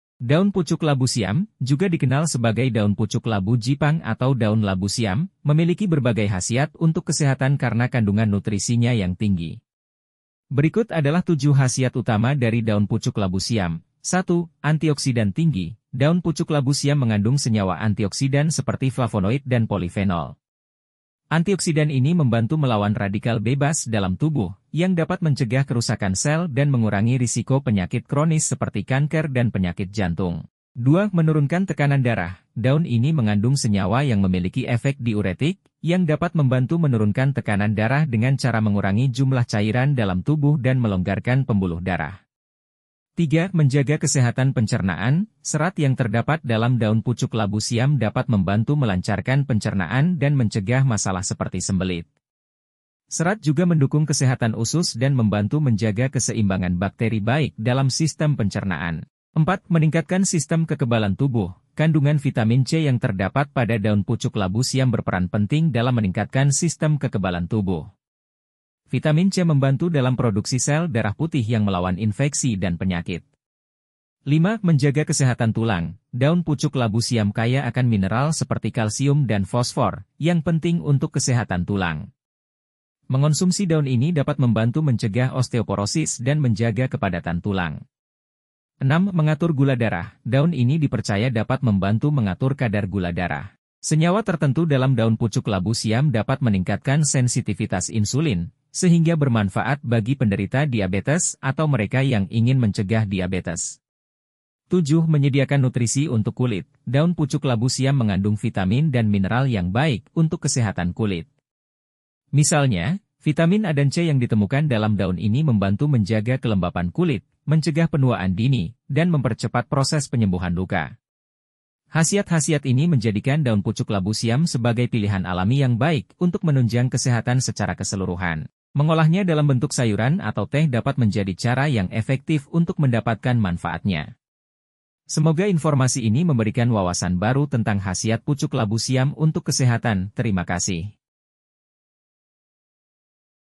7 Khasiat utama pucuk daun labu siam
Pucuk daun labu siam mengandung berbagai vitamin dan mineral, termasuk vitamin C, vitamin A, dan zat besi. Nutrisi ini penting untuk mendukung fungsi tubuh yang optimal, seperti menjaga kesehatan kulit, mata, dan sistem kekebalan tubuh.

Daun pucuk labu siam memiliki tekstur yang lembut dan sedikit renyah, serta rasa yang agak manis. Hal ini menjadikannya bahan yang populer dalam berbagai masakan, terutama di negara-negara Asia Tenggara.